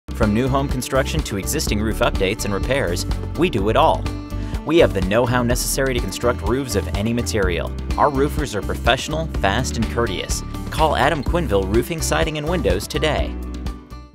Adam Quenneville Roofing, Siding, & Windows
Describe your business's expert residential roofing services with this general ad.

"A house without a roof is barely a house at all.  So make sure yours is installed correctly.
From new home construction to existing roof updates and repairs, we do it all.  We have the know-how necessary to construct roofs of any material, no matter the style of your home.  Our roofers are professional, fast and courteous and will give you a beautiful new roof that will last for years against all kinds of weather.